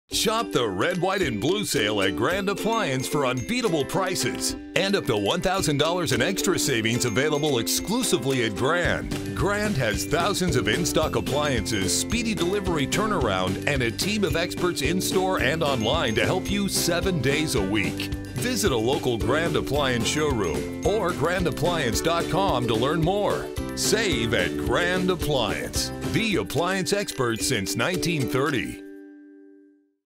Grand Appliance Red White and Blue Sale
Shop the Red White and Blue Sale at Grand Appliance for unbeatable prices and up to $1,000 in additional rebate savings you won't find anywhere else on popular brands like:  @CAFEAppliances ,  @electroluxus ,  @Frigidaire ,  @geappliances ,  @kitchenaid ,  @LGUSA ,  @maytag ,  @SamsungUS  and  @WhirlpoolUSA .

Sale Dates:
6/21/2023 - 7/12/2023

Shop the sale and get full offer details including eligible items and official rebate form and more here:

Need it fast? Grand has thousands of in-stock appliances, speedy delivery, and a team of experts in-store and online to help you 7 days a week.